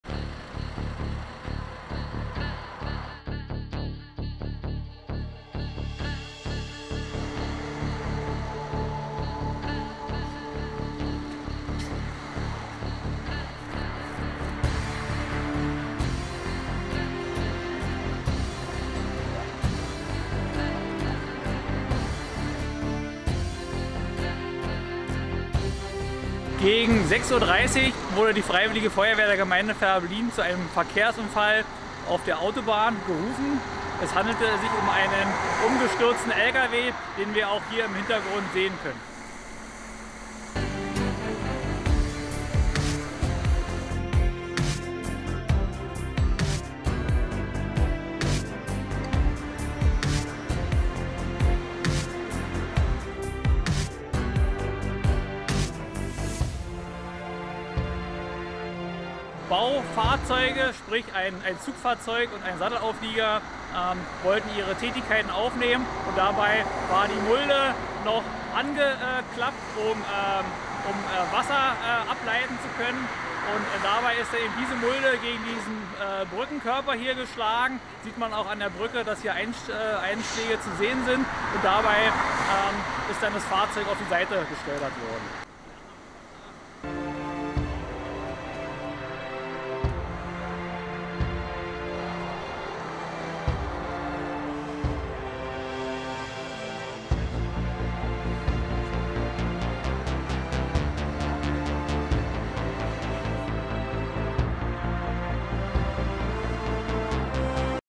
A24 - LKW mit Schüttmulde kracht gegen Autobahnbrücke, Fehrbellin
PM der PD West - Ein 41-jähriger Lkw-Fahrer aus Mecklenburg-Vorpommern fuhr heute Morgen gegen 06.20 Uhr (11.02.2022) mit seinem Lkw-Gespann der Marke Scania und einem ungeladenen Kipperauflieger an der Anschlussstelle Fehrbellin auf die Bundesautobahn 24 in Richtung Berlin auf.

Beim Unterfahren der Brücke (Landesstraße 16) prallte der Auflieger mit der noch aufgerichteten Schüttmulde gegen die Brücke, kam nach links ab, touchierte den Brückenmittelpfeiler und auf der Fahrbahn seitlich zum Liegen. Die Richtungsfahrbahn in Fahrtrichtung Berlin musste für mehrere Stunden gesperrt werden und dauerte bis zirka 13.45 Uhr an. Eine Ableitung des Verkehrs erfolgte an der Anschlussstelle Neuruppin-Süd.

Für eine Einschätzung der Tragfähigkeit der Brücke kam ein Statiker zum Einsatz. Die L 16 konnte dauerhaft befahren werden. Der 41-jährige Lkw-Fahrer wurde bei dem Unfall leicht verletzt und anschließend in ein Krankenhaus gebracht. Die Schadenshöhe wird mit zirka 100.000 Euro angegeben.